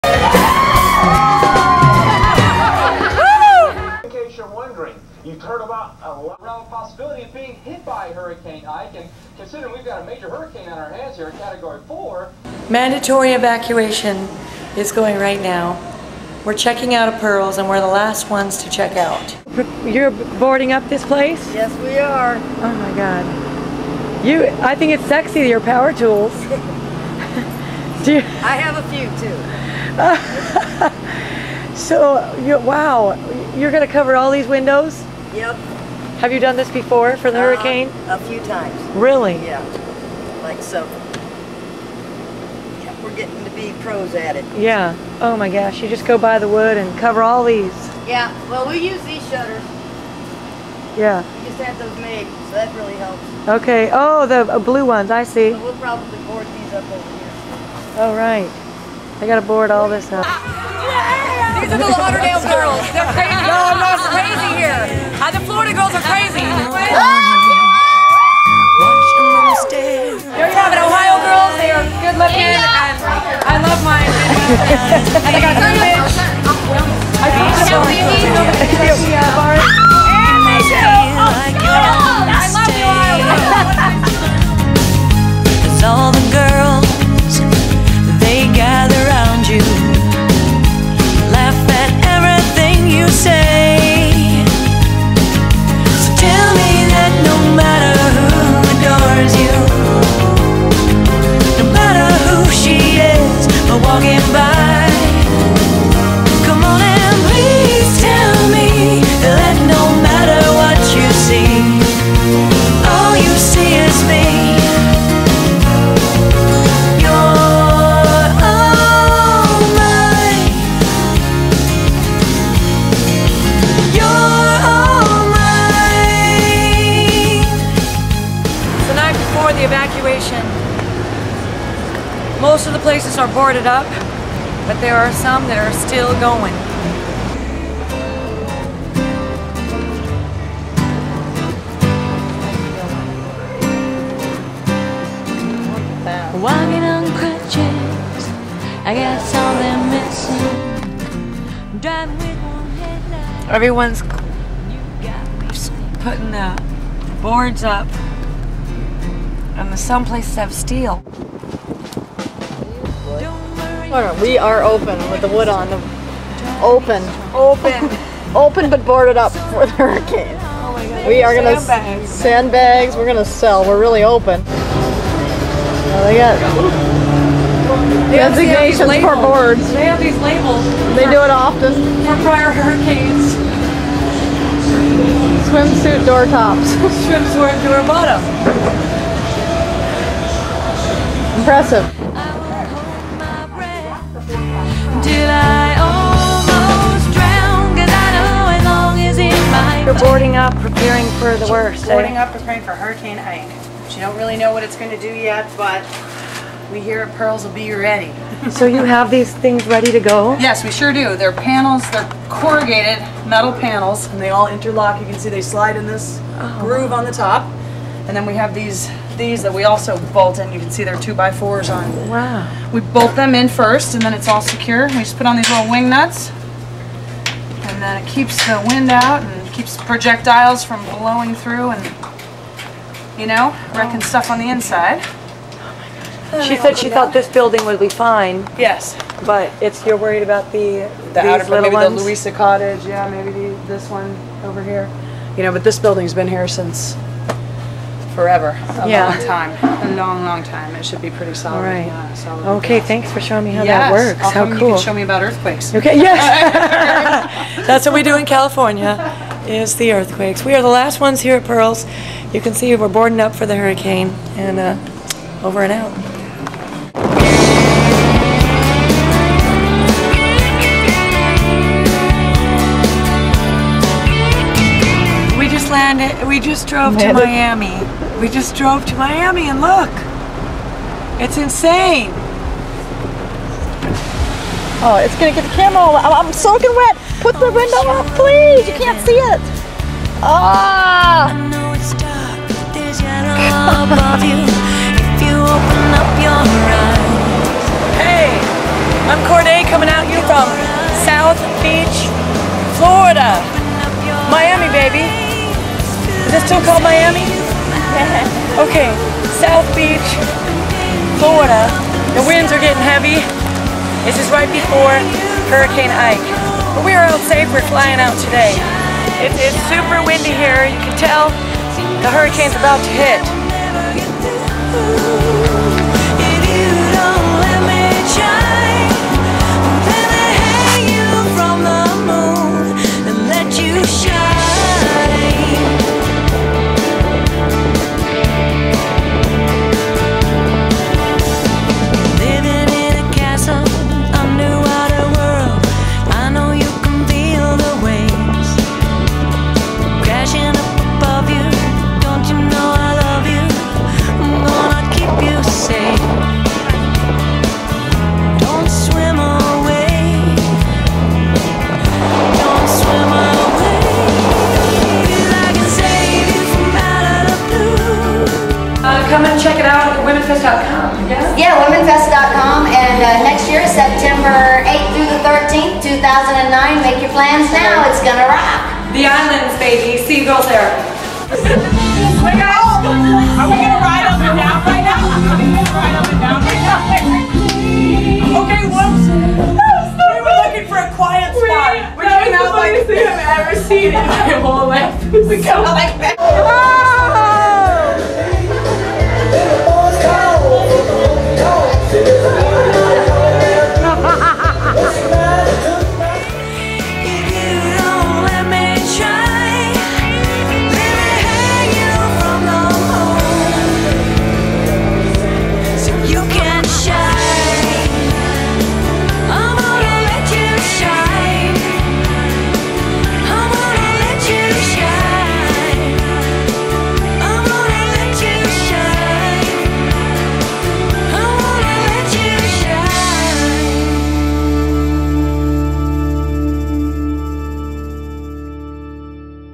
CORDAY'S GIRL ROCK Episode 12, Part 3: Key West Evacuation!
KEY WEST III: The Evacuation
Florida women bear all at the final farewell pool party at Pearl's.  Corday entertains before Hurricane Ike mandates the whole town evacuate!